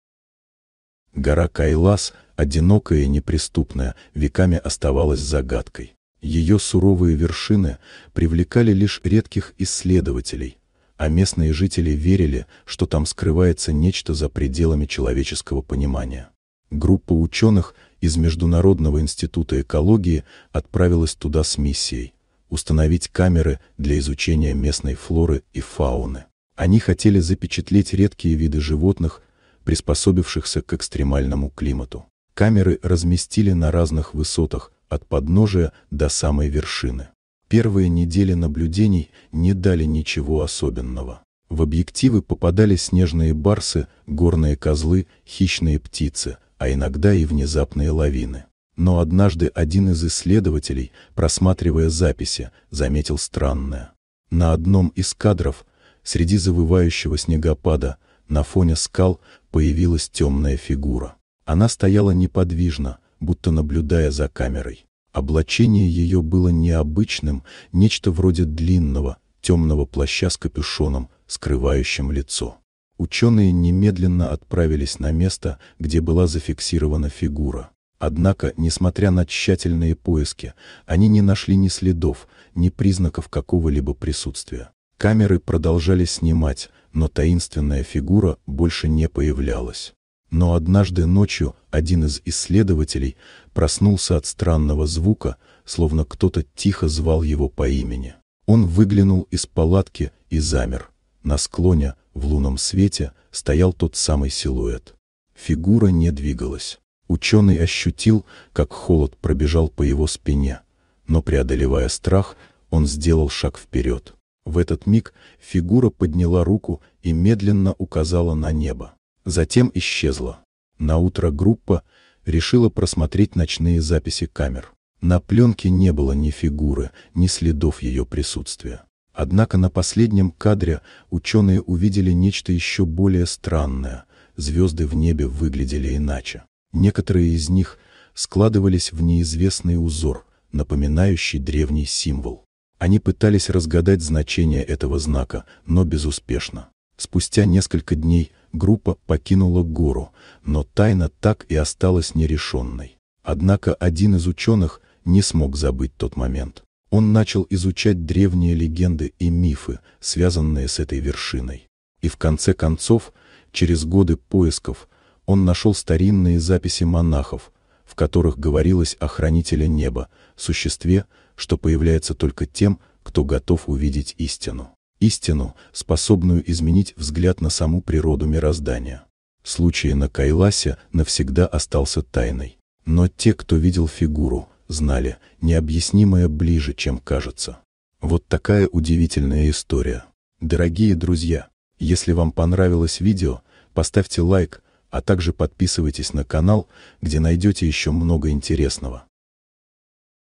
Учёные установили камеры в горах. Увиденное шокировало весь мир!
Гора Кайлас, одинокая и неприступная, веками оставалась загадкой. Её суровые вершины привлекали лишь редких исследователей, а местные жители верили, что там скрывается нечто за пределами человеческого понимания. Группа учёных из Международного института экологии отправилась туда с миссией: установить камеры для изучения местной флоры и фауны. Они хотели запечатлеть редкие виды животных, приспособившихся к экстремальному климату. Камеры разместили на разных высотах — от подножия до самой вершины.